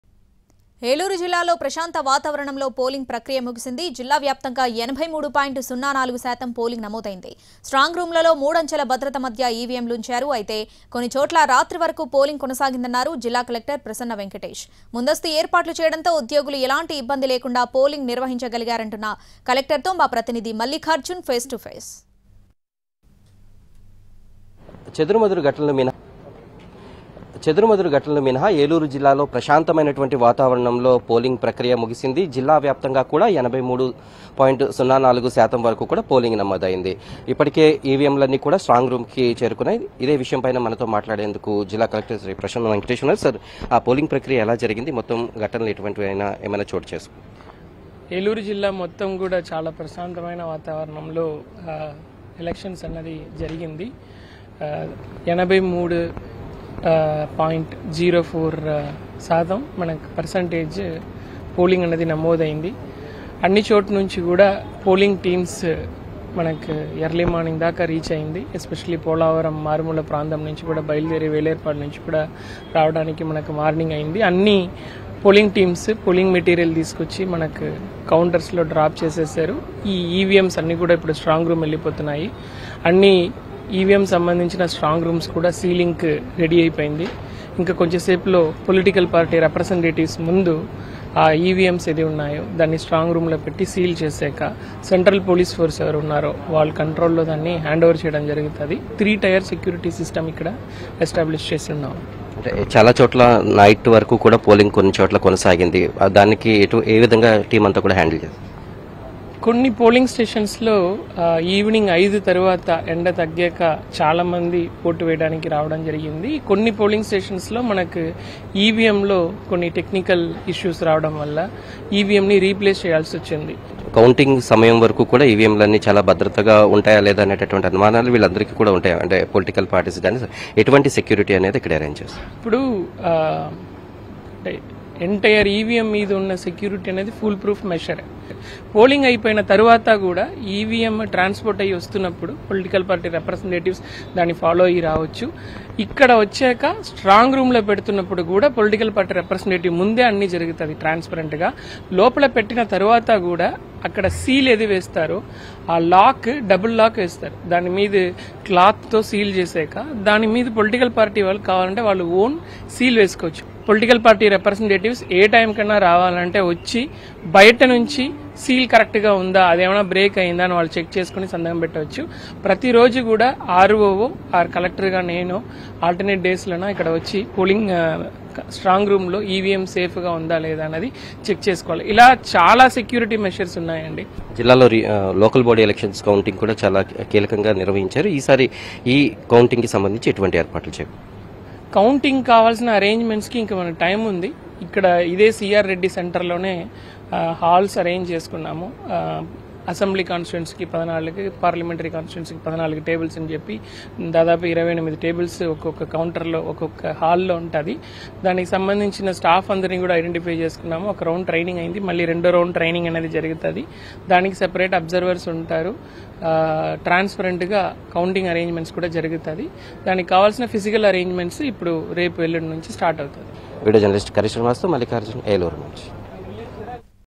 83.04% Turnout Recorded in Eluru District | Special Report On AP Elections 2024 | Ntv
For more latest updates on the news :
83.04% Turnout Recorded in Eluru District | Special Report On AP Elections 2024 | Ntv